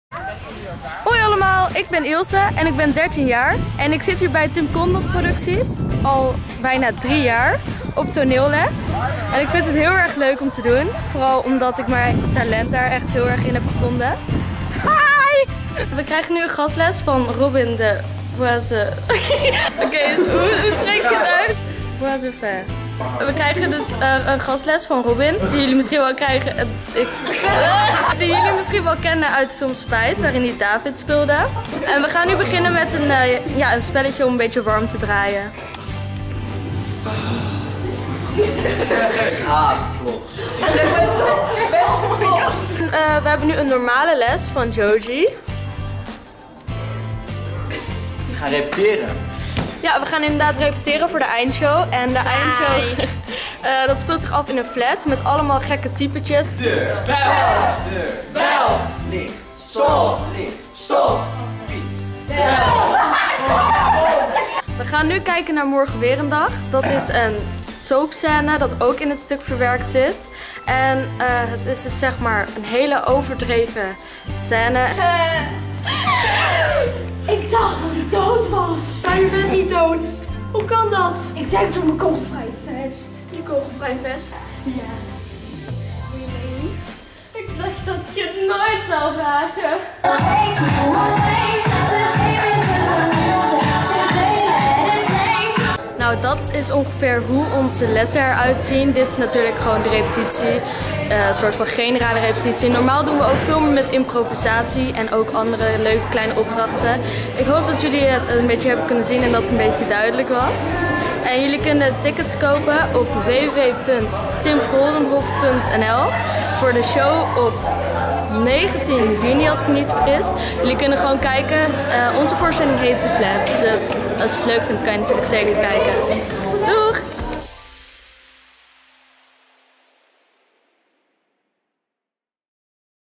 Een kijkje bij de Theaterles! | TKP Theater Vlog #5
De eindshows van TKP komen eraan! De komende tijd zullen er 10 vlogs op Youtube verschijnen, geheel gefilmd door de leerlingen zelf. Wie zijn ze, en waar zijn ze zoal mee bezig? In de vlogs zie je hoe het er in de repetities en in de studio’s aan toe gaat. Met persoonlijke verhalen, een uniek kijkje achter de schermen!